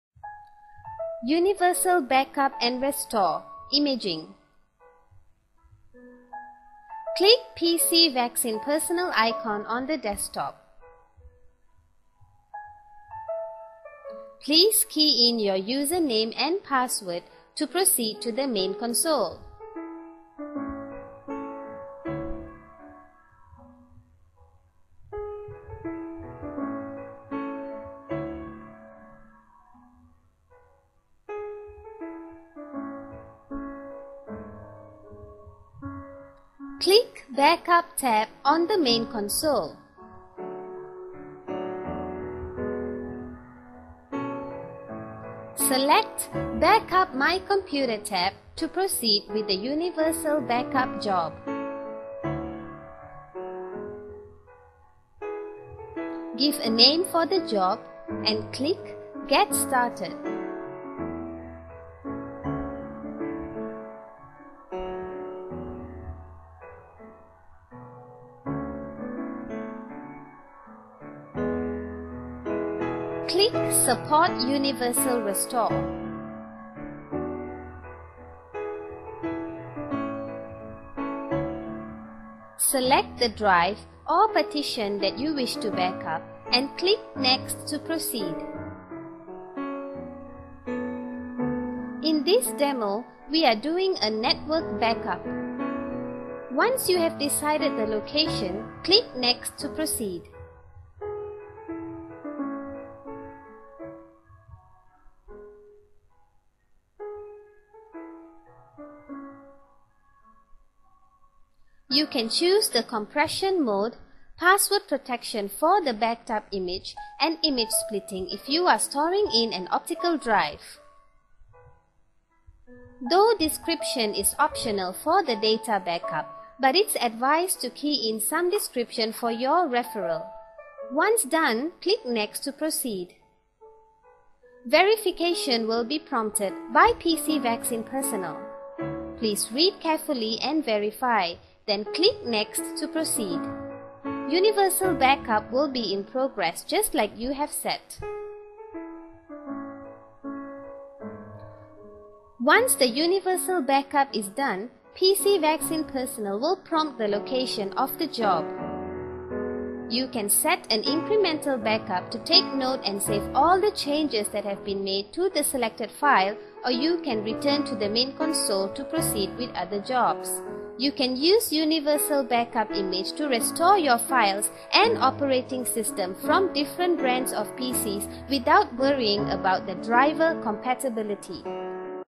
Complete Backup - Universal Restore support
This feature a llow you restore files or images across different hard drives, regardless of hard drive format, size, brand, or operating system